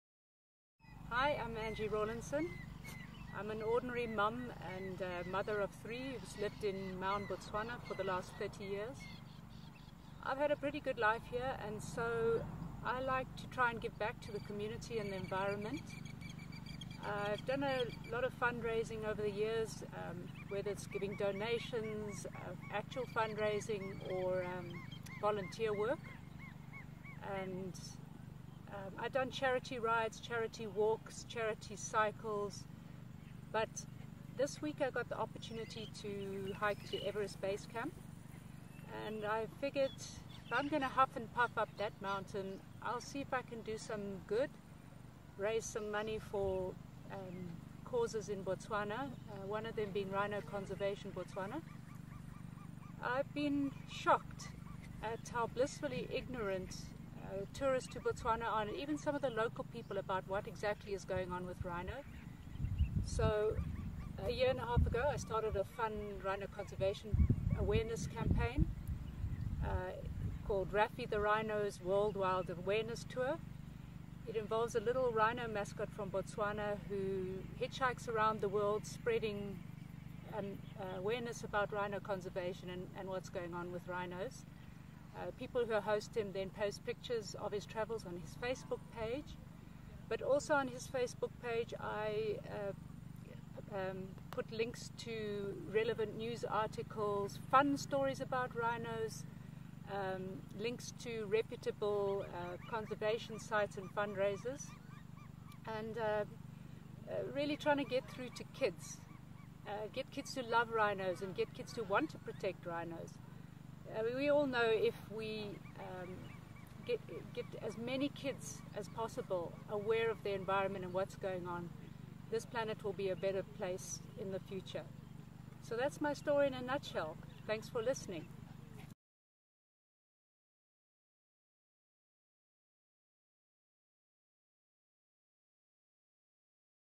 Support Angie's big hike to Everest for rhinos
Rhino Conservation Botswana (RCB) supporter & fundraiser Angie Rawlinson is heading to Mount Everest Base Camp and is asking you to support her epic journey by making a donation to RCB